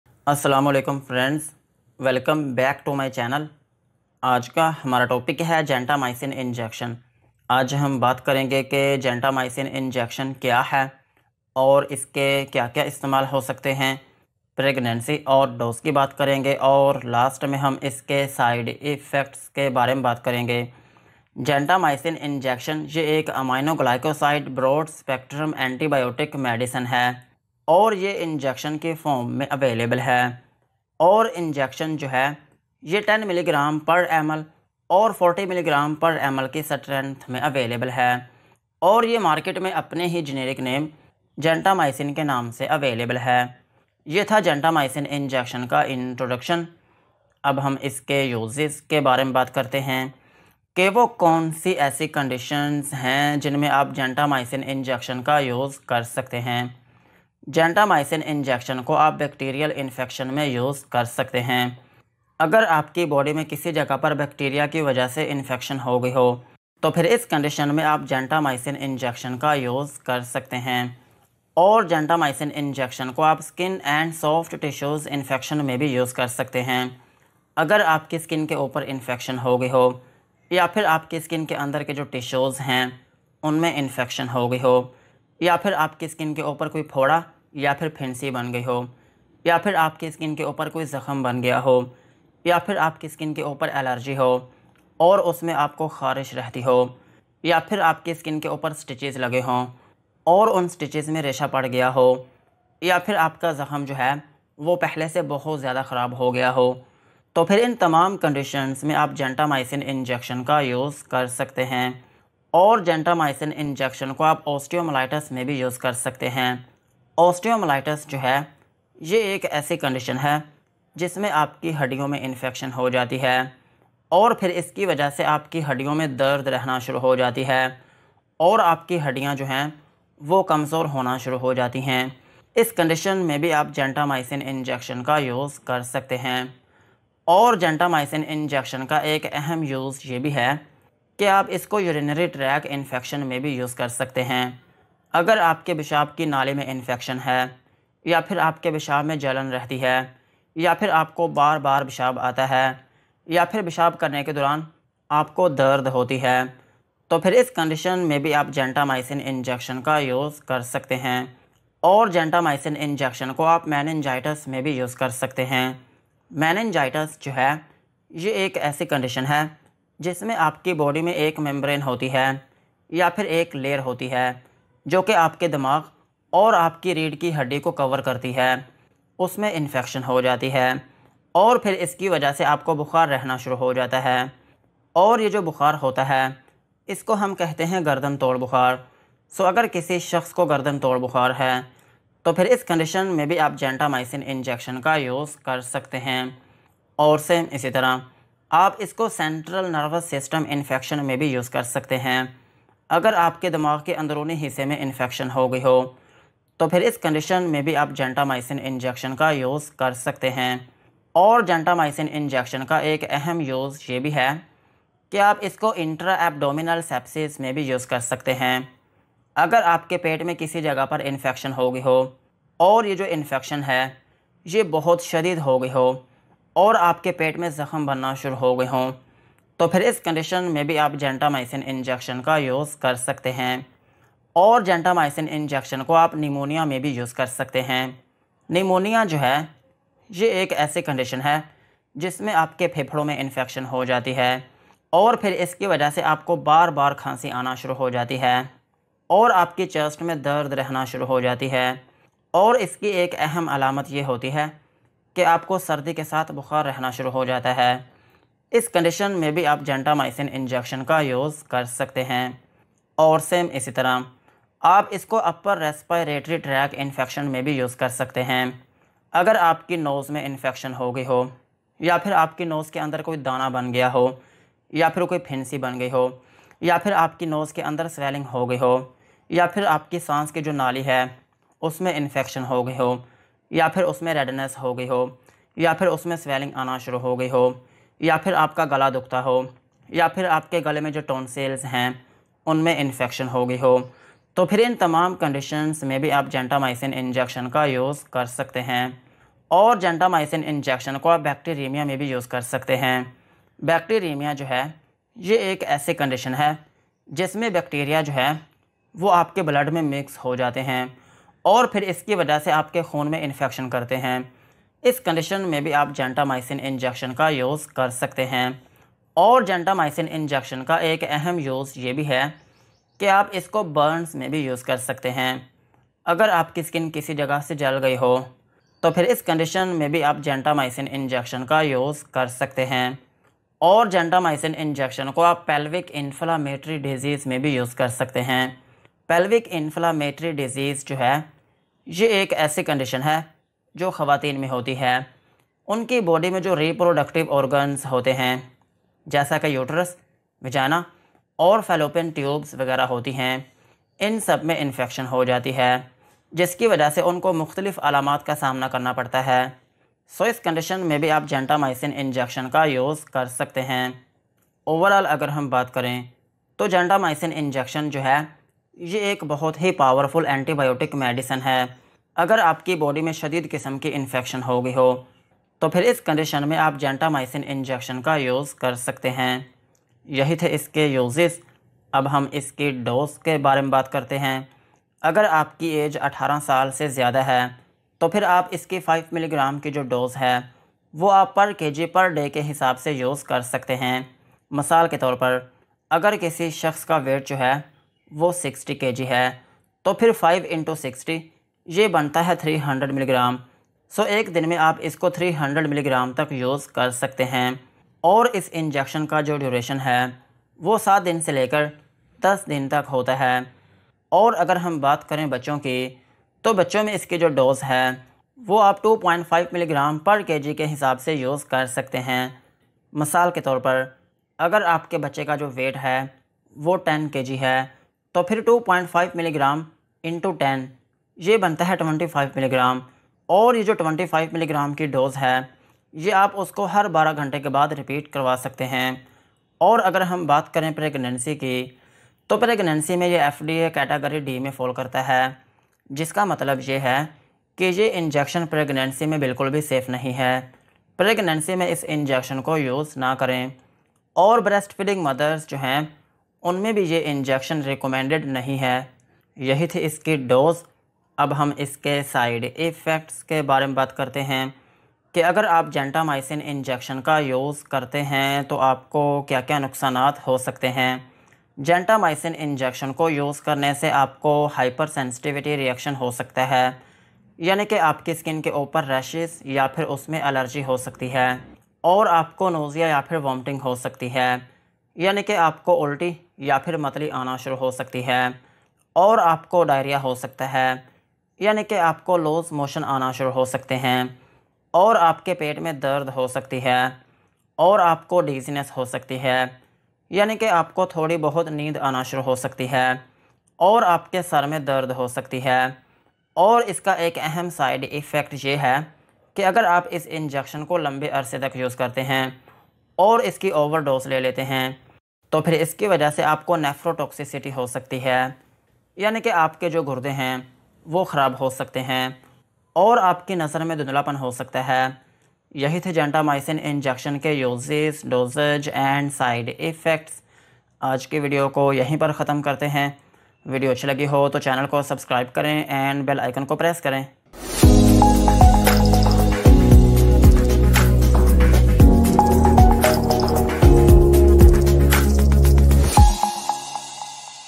Gentamicin Injection Uses | Gentamicin Injection Ke Fayde | Gentamicin Injection Side Effects
🔥 Key Points Covered:

✅ Introduction
✅ Available forms
✅ Strengths
✅ Uses in detail
✅ Precautions
✅ Adult dose
✅ Pediatric dose
✅ Pregnancy
✅ Warnings
✅ Side effects

Gentamicin injection is a broad spectrum aminoglycoside antibiotic medicine which is available in the form of injection . Gentamicin injection is available in 10mg/ml, 40mg/ml and 8mg/ml strengths. Gentamycin injection can be used to treat bacterial infections in our body including both gram-positive and gram-negative bacteria. Gentamicin injection can be used  in serious infections caused by bacteria.
Fore more details please watch my video so you can get more details about this medicine.